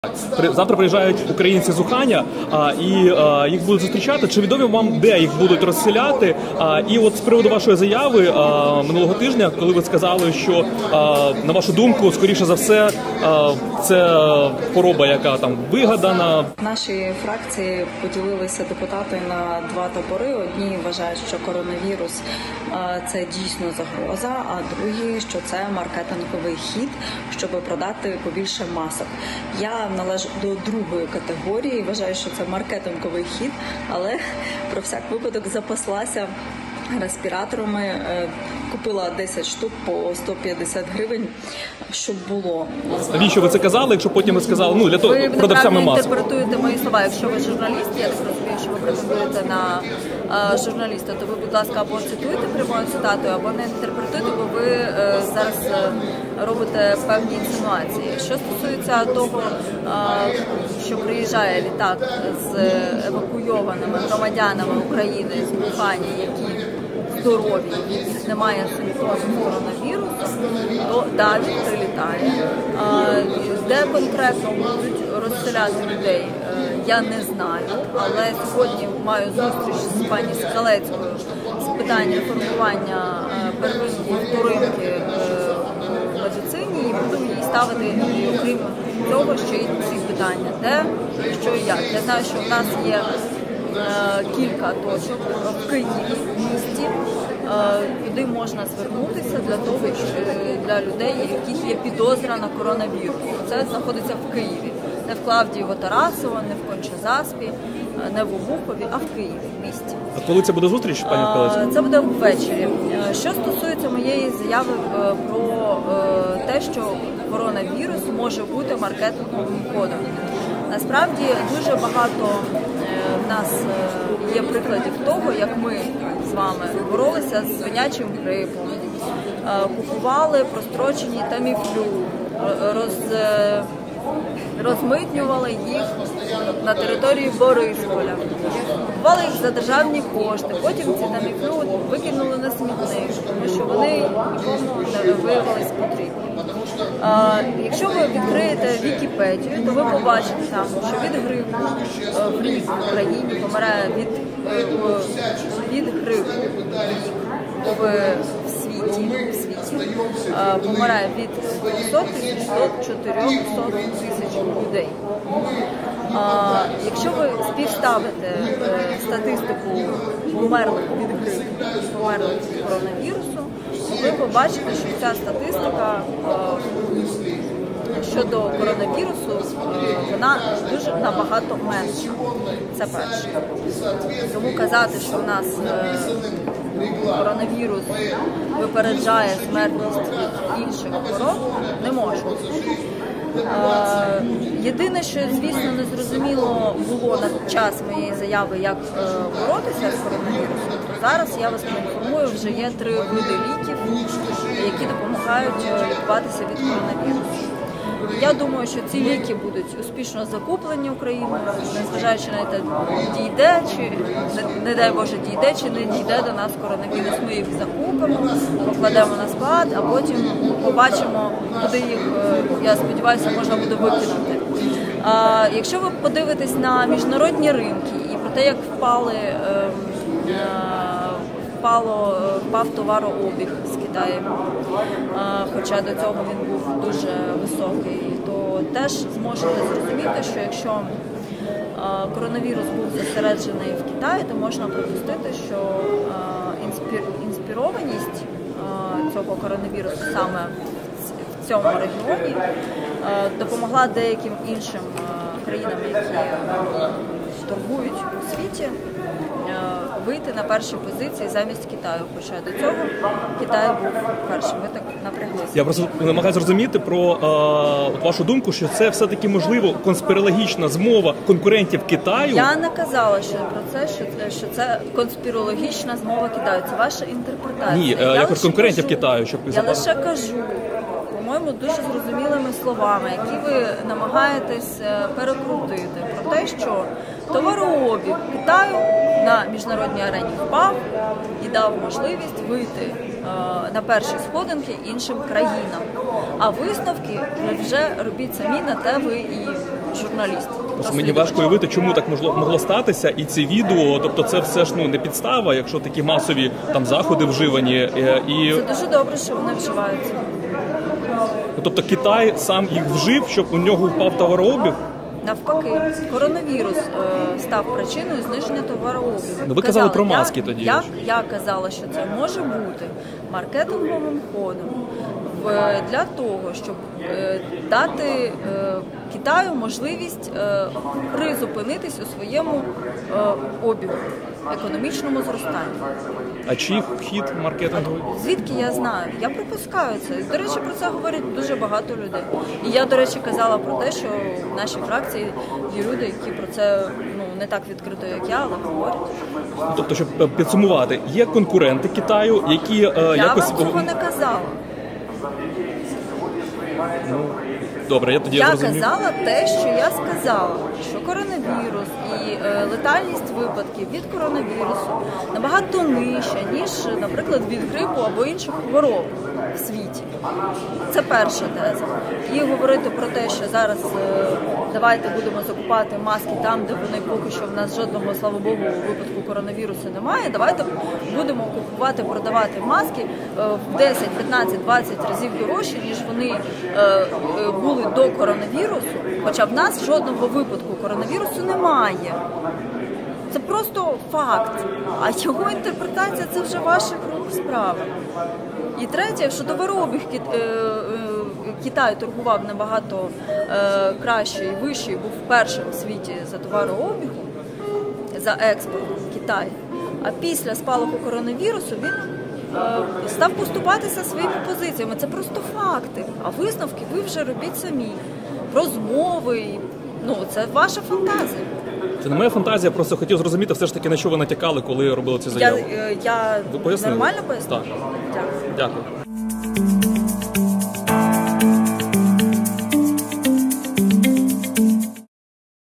"Коронавирус - маркетинговый ход". Депутат Василевская объяснила свои высказывания о вирусе
На прошлой неделе депутат "Слуги народа" Ольга Василевская назвала эпидемию коронавируса "маркетинговым ходом".

Нардеп отметила, что в их фракции есть две группы: те, кто считают коронавирус реальной угрозой, и те, кто думает, что это маркетинговый ход, чтобы продать побольше масок.

"В нашей фракции поделились депутаты на два лагеря: одни считают, что коронавирус - это действительно угроза, а другие - что это маркетинговый ход, чтобы продать побольше масок. Я принадлежу ко второй категории, считаю, что это маркетинговый ход, но на всякий случай запаслась респираторами, купила 10 штук по 150 гривен, чтобы было", - сказала нардеп.

Мы решили уточнить у Ольги Василевской, что именно она имела ввиду под словосочетанием "маркетинговый ход"